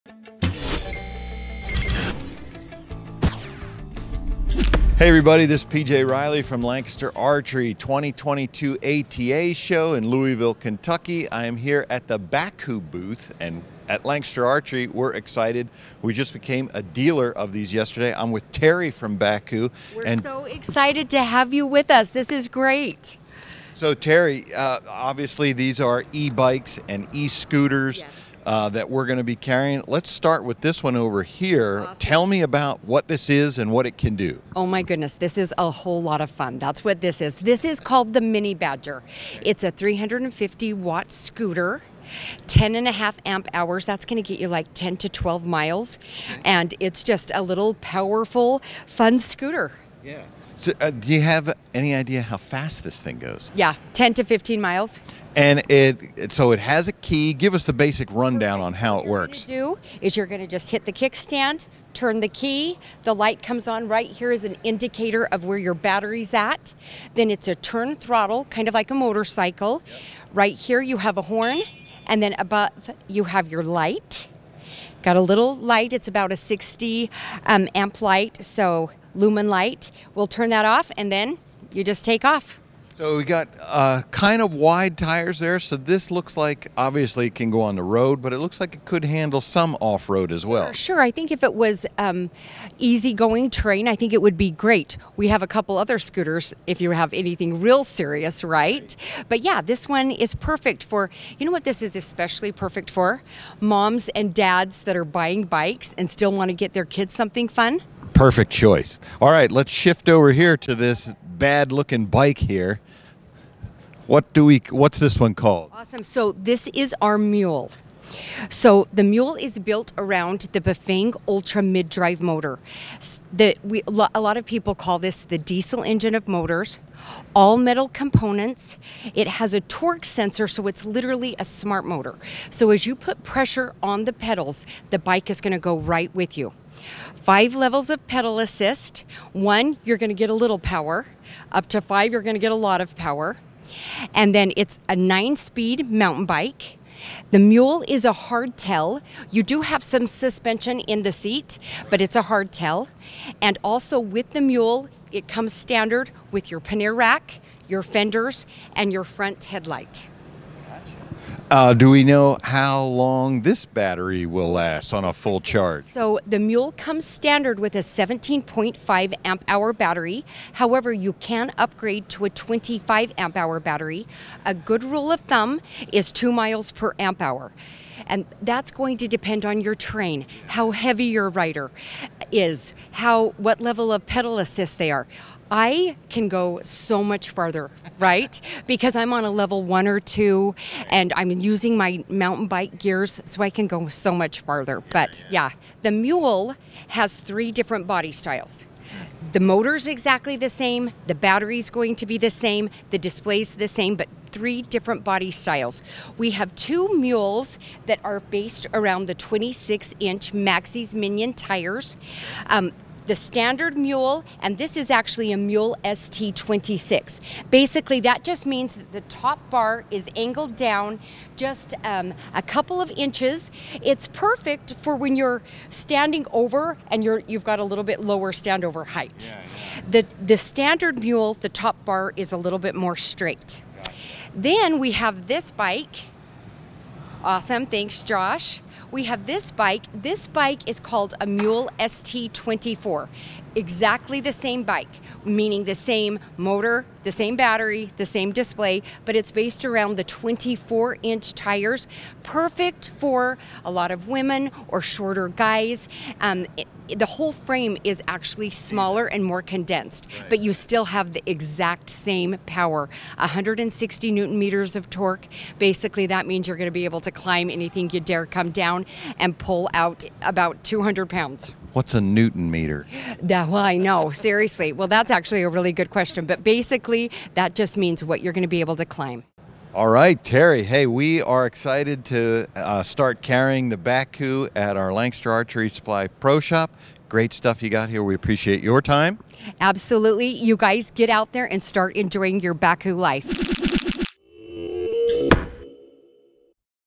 2022 ATA Show | Bakcou eBikes
New to Lancaster Archery Supply is Bakcou eBikes! They have two different styles of bikes- a scooter and a mountain bike. The Badger Scooter can run for 10-12 hours, it can go 10-15 mph, and would be perfect on easy terrain. For the more serious biking, Bakcou has the Mule Mountain Bike. This bike has a powerful Bafang motor; it's a 9-speed bike; and it has a rack, fenders, and light. The Mule also comes in three different styles, so it will fit any rider! Make sure to check our website out at lancasterarchery.com for more information as we are excited to get these bikes in stock!